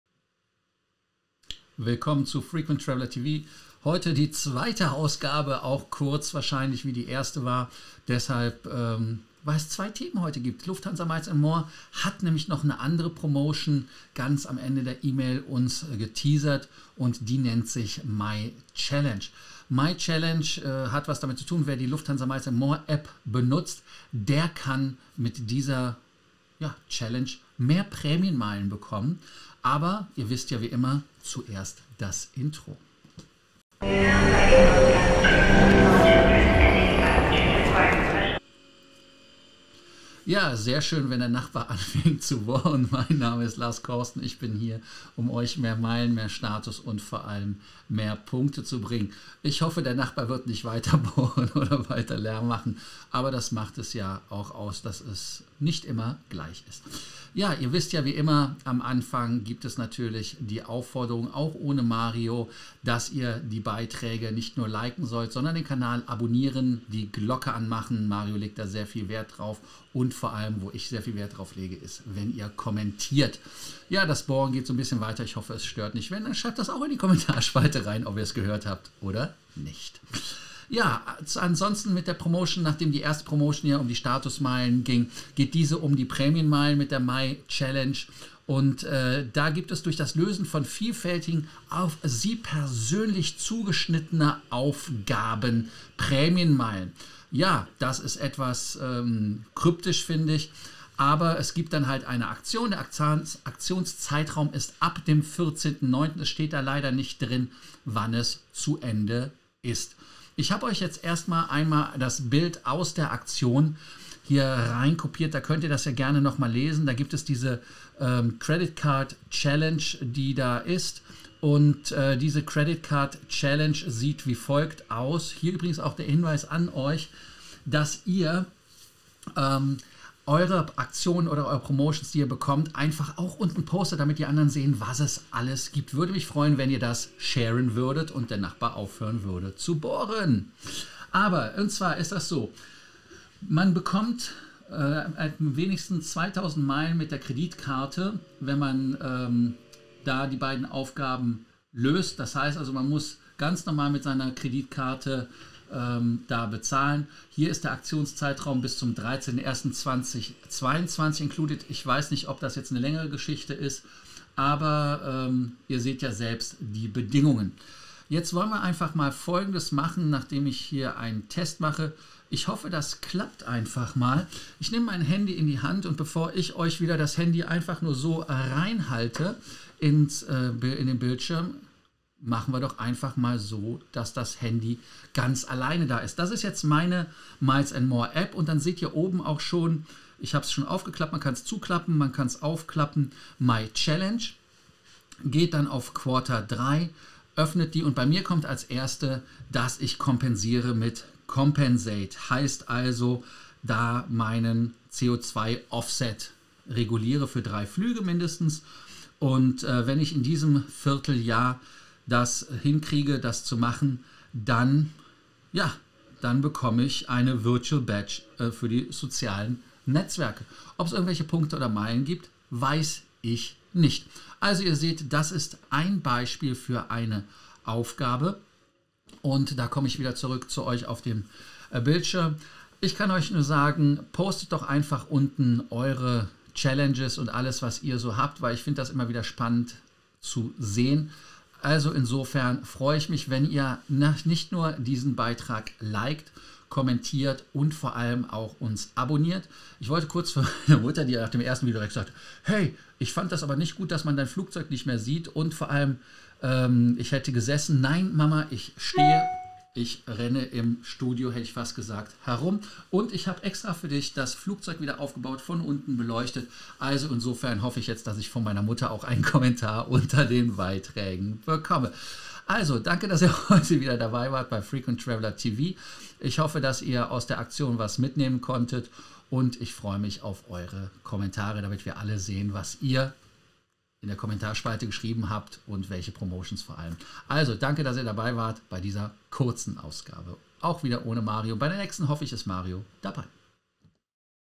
Lufthansa Miles & More MyChallenge
Mit kleinen, individuellen Aufgaben zusätzliche Prämienmeilen sammeln

Mit "MyChallenge", einem neuen Angebot in der Miles & More App, erhalten Teilnehmer die Möglichkeit, durch das Lösen von vielfältigen und auf sie persönlich zugeschnittenen Aufgaben entweder mehrfache Prämienmeilen oder einen höheren Meilenbetrag bei Zielerreichung als üblich bei Miles & More Partnern zu sammeln. Spielerisch und ganz nebenbei.

Prämienmeilen werden dem jeweiligen Meilenkonto direkt nach Abschluss einer Challenge gutgeschrieben. Wer nimmt die Herausforderung an?

Aktionszeitraum: ab 14.09. dauerhaft wechselnde Challenges

Clubhouse - @LFCorsten